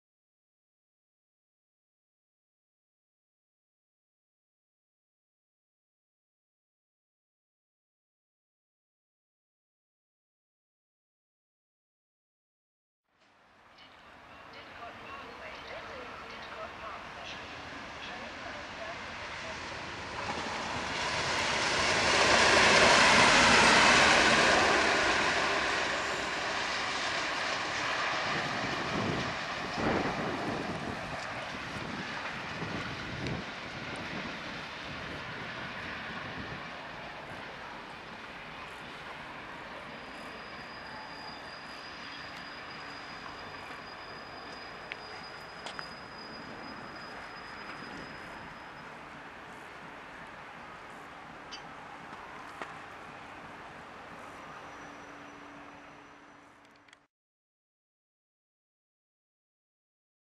DRS' 37218 on 0Z40 @ Didcot Parkway 2/9/10
Here we see Direct Rail Services 37218 passing Didcot Parkway's station avoiding line at 11:22 dead on time working (0Z40) 07:40 Crewe Gresty Bridge - Eastleigh Rail Centre on the 2nd September 2010. Enjoy!